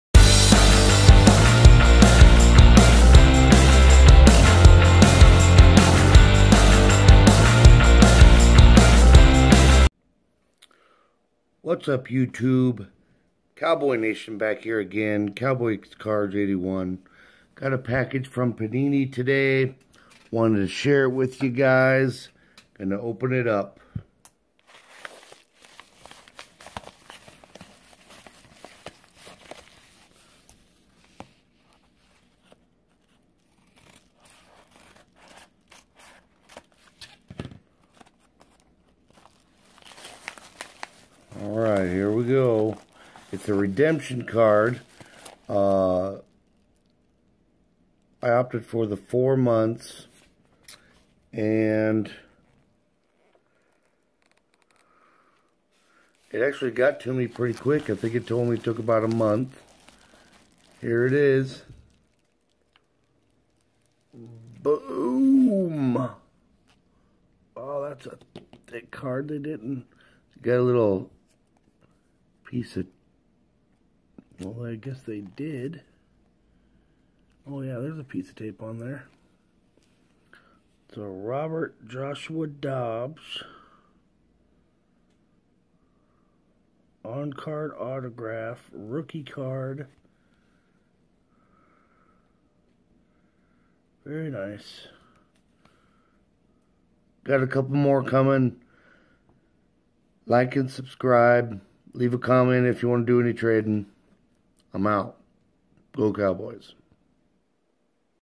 Panini Limited redemption card
Turned in a Redemption card got  back a month later. Please comment what do you think of the card.